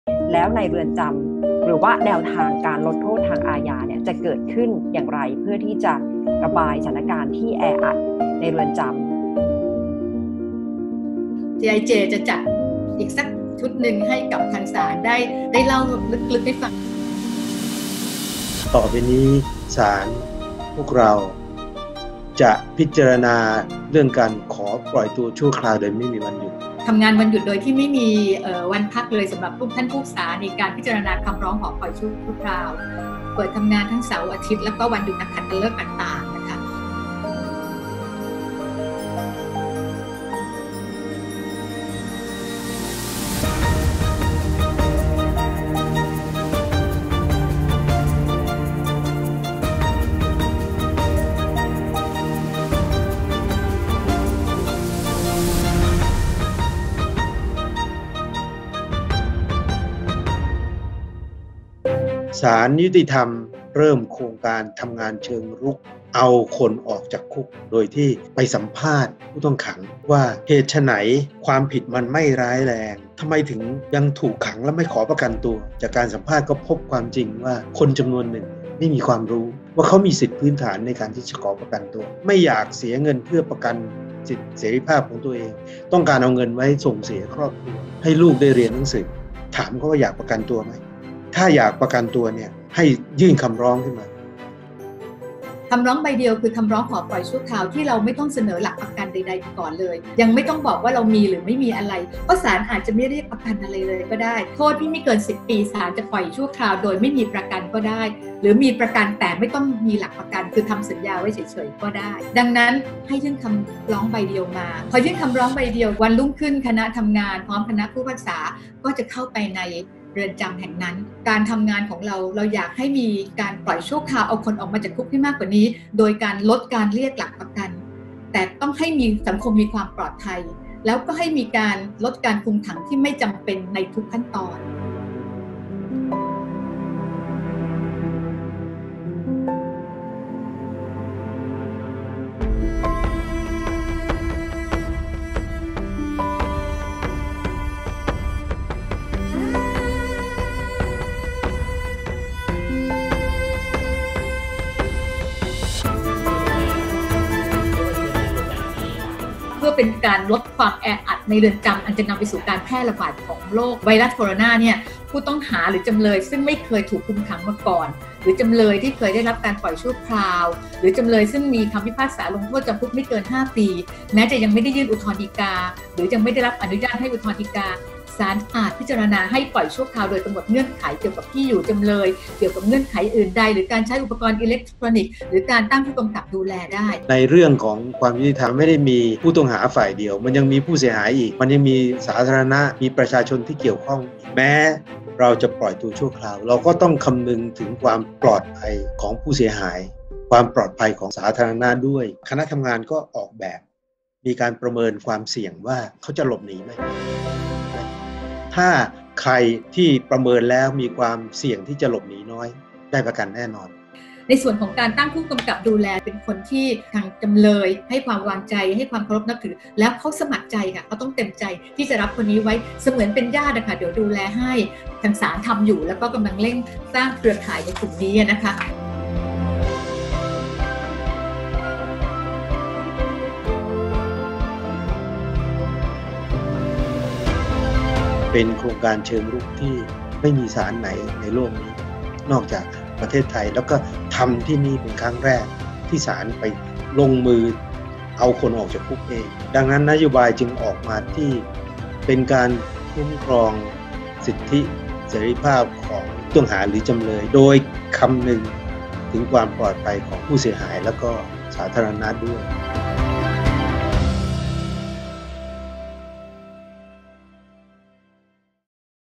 คนจนติดคุกเพราะอะไร ? ศาลไทยยุคโควิดช่วยเหลือประชาชนได้จริงหรือไม่ ? ติดตามได้ในคลิปนี้
“โครงการเชิงรุก ผู้พิพากษาเข้าไปสัมภาษณ์ผู้ต้องขังในเรือนจำ เพื่อหาคำตอบที่ผู้ต้องขังความผิดไม่ร้ายแรง ผู้ต้องขังที่อยู่ระหว่างการพิจารณาคดี ทำไมต้องถูกขัง ไม่ขอประกันตัว ... ศาลไทย เป็นศาลเดียวในโลก ลงมือเอาคนออกจากคุกเอง”

คำกล่าวของ ท่านไสลเกษ วัฒนพันธุ์ ประธานศาลฎีกา เน้นย้ำนโยบายคุ้มครองสิทธิเสรีภาพของผู้ต้องหา ที่คำนึงถึงความปลอดภัยของผู้เสียหายและสาธารณะ

ความผิดเดียวกัน คนรวยได้ประกันตัว คนจนติดคุก เป็นที่มาของ “คำร้องใบเดียว” กระบวนการยื่นคำร้องขอปล่อยชั่วคราวรูปแบบใหม่ ที่เปลี่ยนจาก “การใช้เงินเพื่อประกันตัว” เป็น “การประเมินความเสี่ยงหลบหนี” แก้ปัญหาความเหลื่อมล้ำ ไม่มีใครควรติดคุกเพราะความจน

ติดตามรายละเอียดของท่านไสลเกษ วัฒนพันธุ์ ประธานศาลฎีกา และ ท่านเมทินี ชโลธร รองประธานศาลฎีกา ส่วนหนึ่งจากงาน RoLD Forum: Living with COVID-19 ตอนที่ 5 "วิถีใหม่ศาลยุติธรรมยุคโควิด-19" เมื่อวันที่ 16 พฤษภาคม 2563